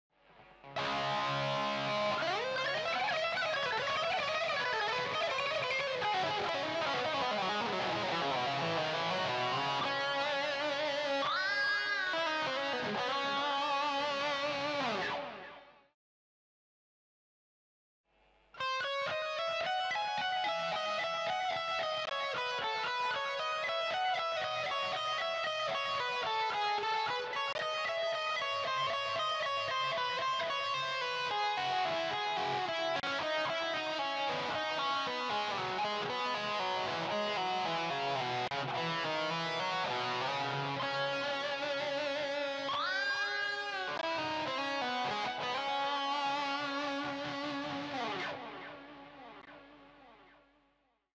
Advanced Rock Guitar Lick - Alternate Picking and Legato
This is my first video of a guitar lick series that I'm trying out. This particular lick has an alternate picking pattern (included the harmonic minor note) followed by a descending legato run using finger barring as well and ending with a short phrase.